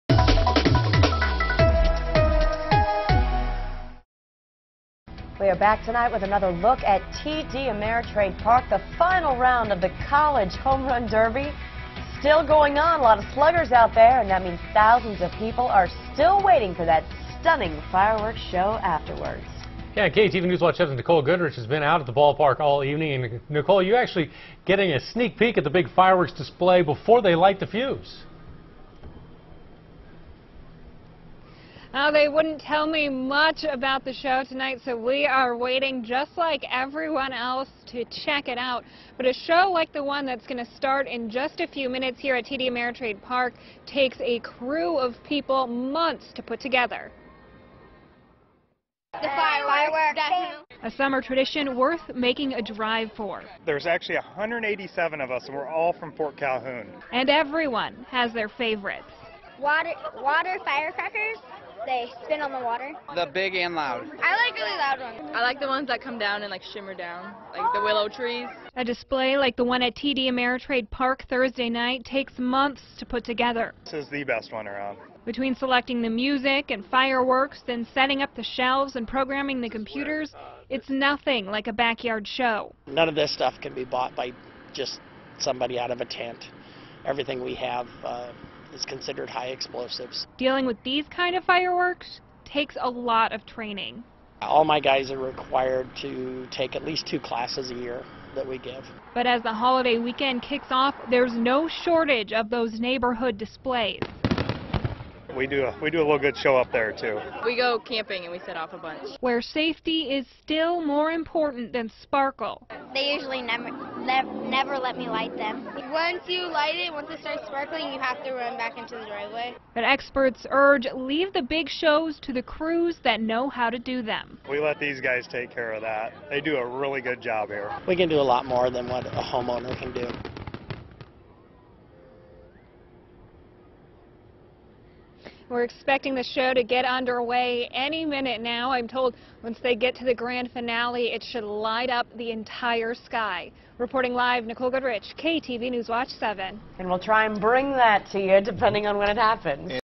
TD Ameritrade lights up the sky Thursday night
We are getting a behind-the-scenes look for Thursday’s fireworks show at TD Ameritrade Stadium.